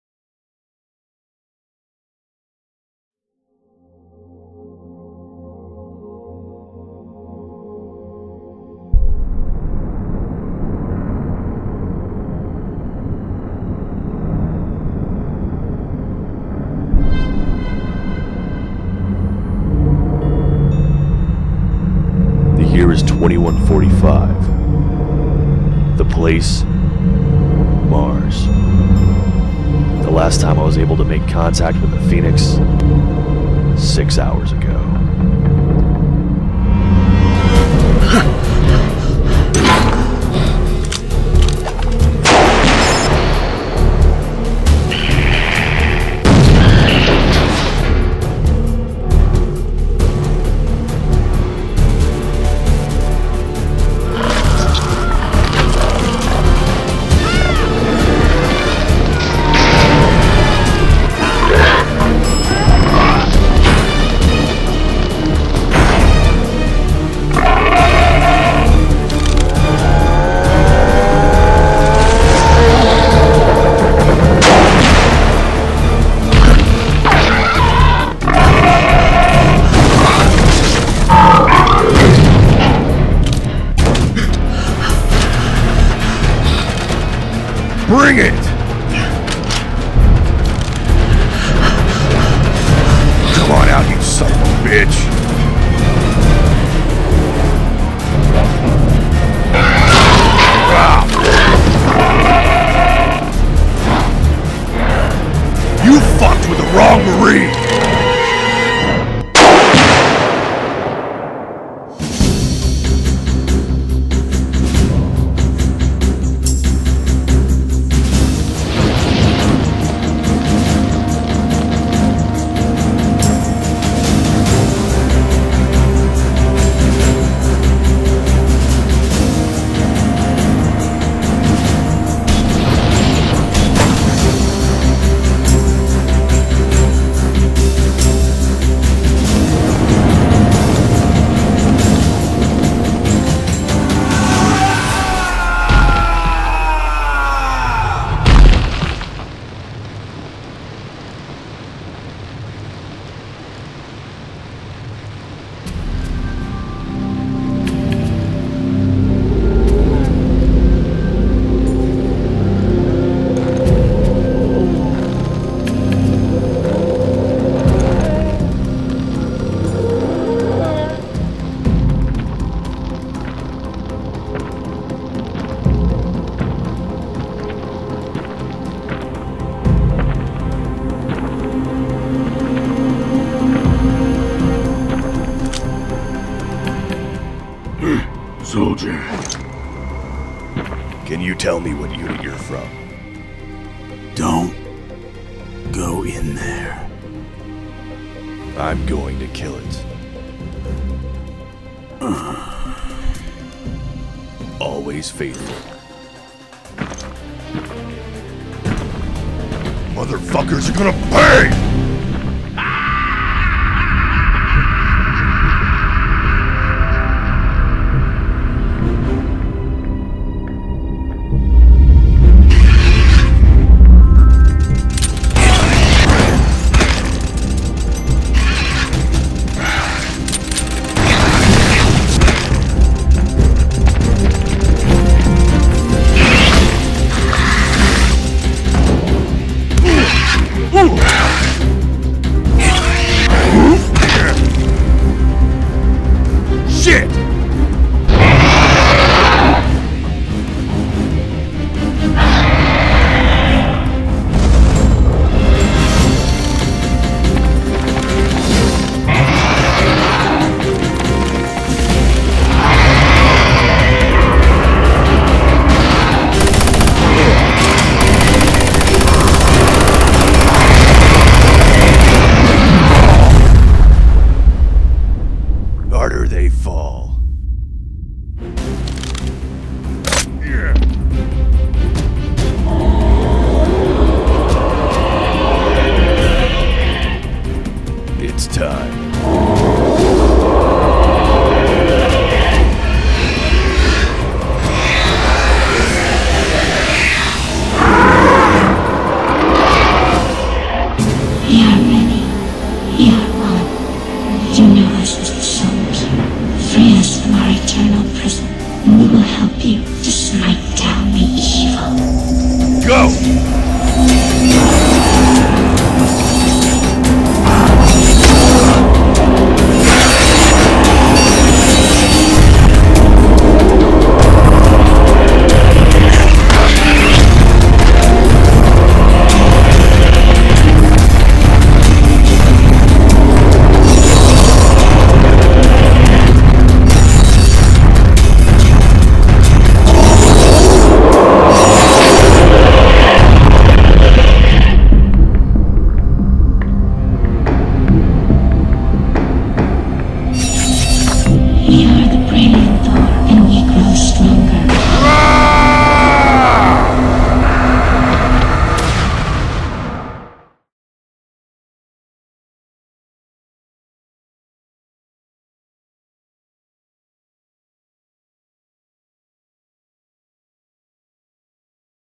Episode 0 - Deleted and Unfinished Scenes 1 - Very Rare
This is the fully rendered Episode 0 original with the complete ending of Doom taking down the Cyberdemon.   This is about the rarest video I have dating back to 2007 of the project.   This version never saw Youtube and most of the Staff never saw the final.